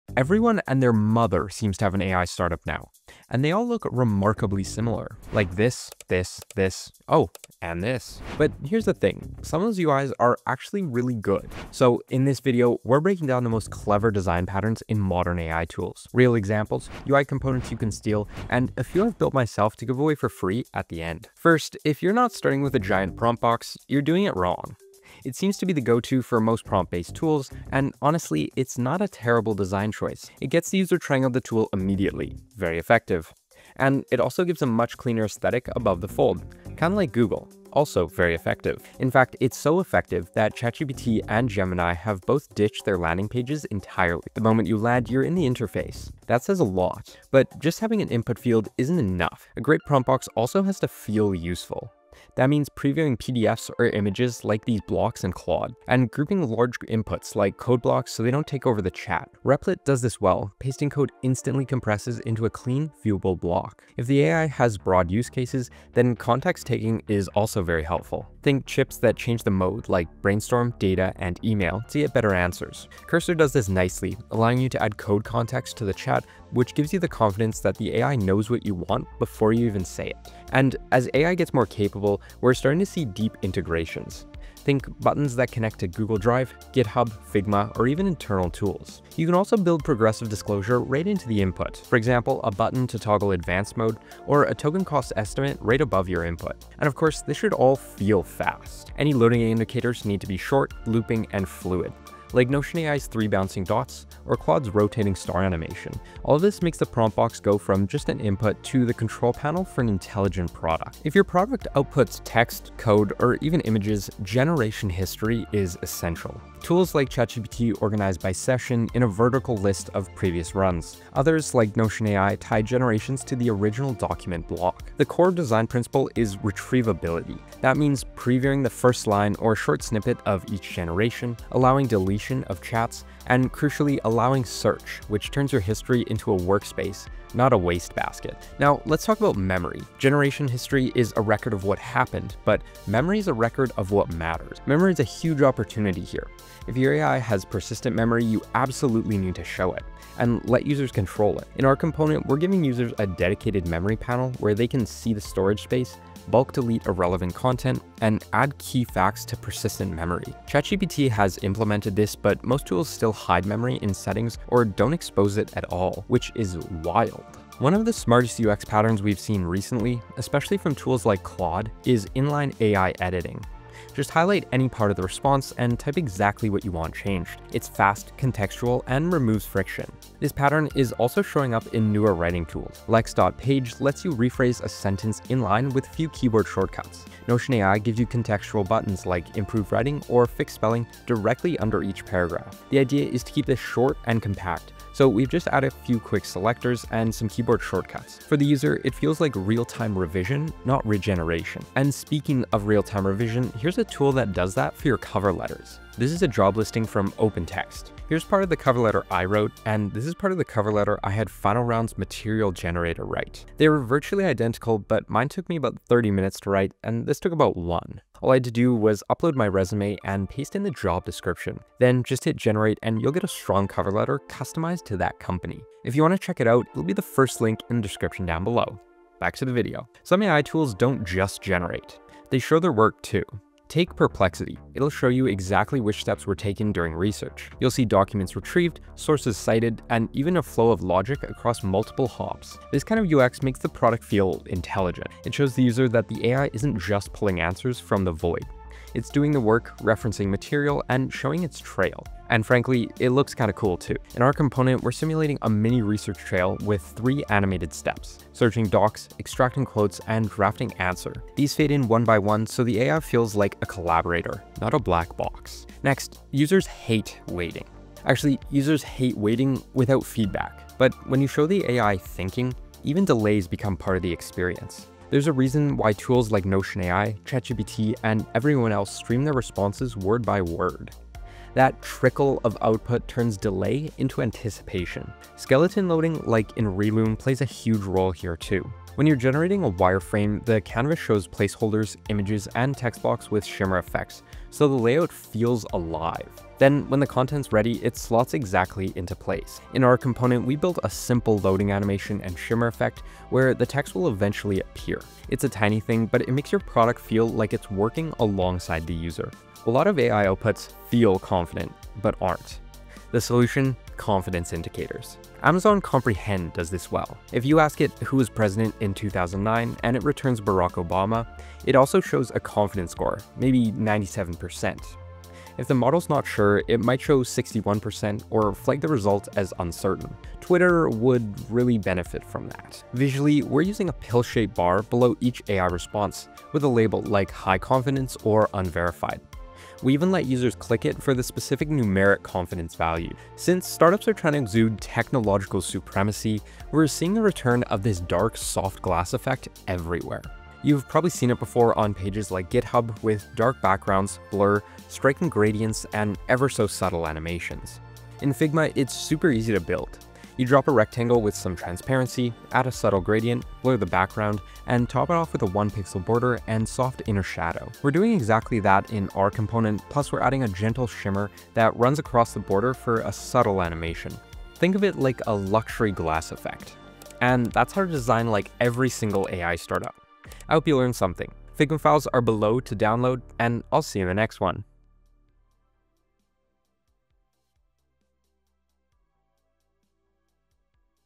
The 7 UI Components to Design Like Unicorn AI Startups
We're breaking down the 7 UI components Unicorn (billion dollar) AI startups always seem to be using so you can copy their UX approach, and so you can use these clean designs in your projects. Enjoy!

Timestamps:
0:00 Intro
0:21 Prompt Boxes
1:51 Generation History / Memory
2:48 Inline Prompting
3:50 AI Deep Research
4:26 Real Time Display
5:10 AI Confidence Meters
5:43 Soft Glass UI